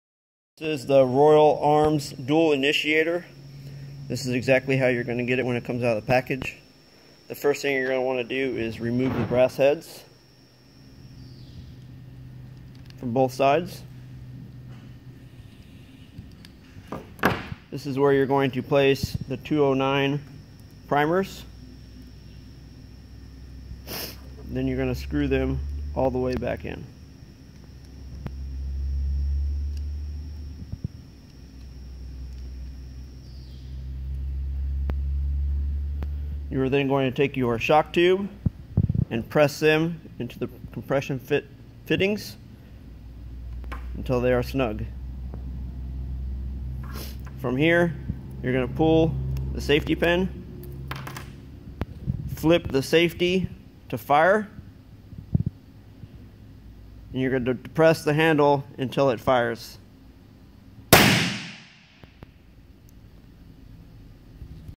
Royal Arms RDI Dual Shock Tube Initiator part1
Royal Arms RDI Dual Shock Tube Initiator part1 in this video it shows how to Arms and Fire the RDI